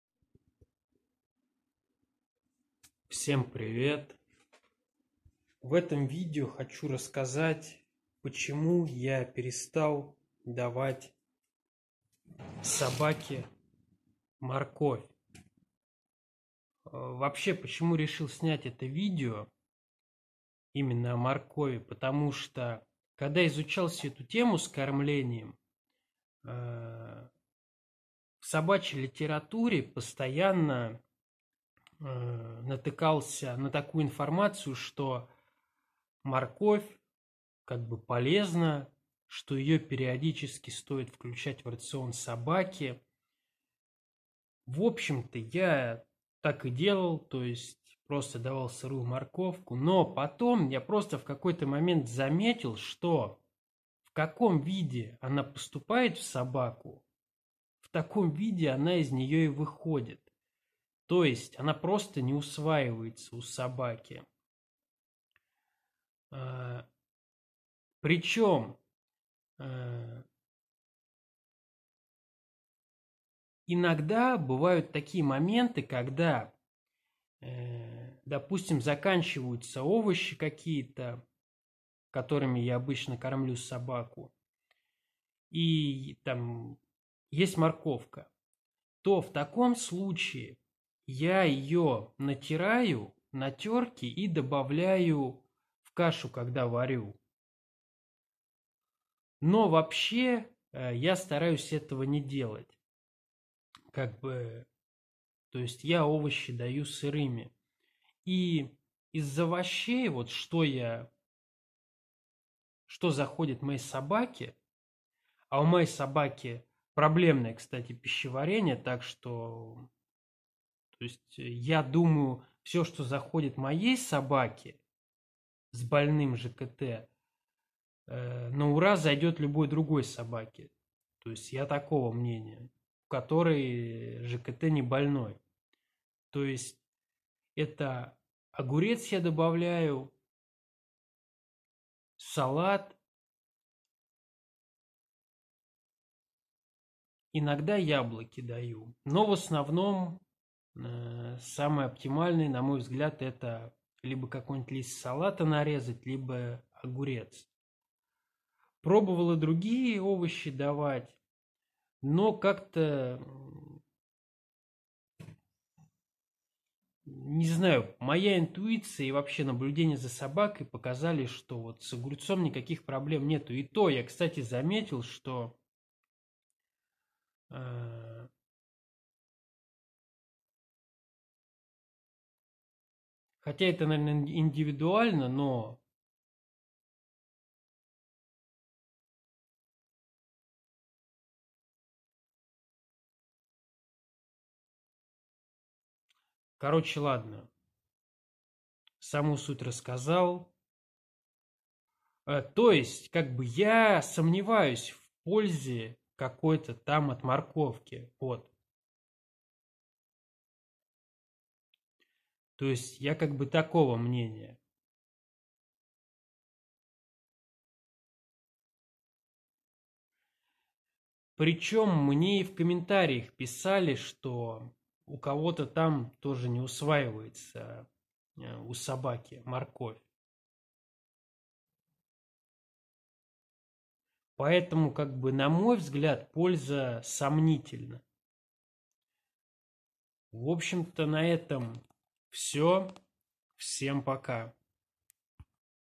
Почему я перестал давать амстаффу морковь
Очень часто натыкался в интернете на статьи в которых рекомендуют периодически давать собаке морковь но покормив какое то время собаку морковью я засомневался в её пользе для собаки и в этом видео я расскажу почему я считаю что этот овощ не подходит для кормления собаки.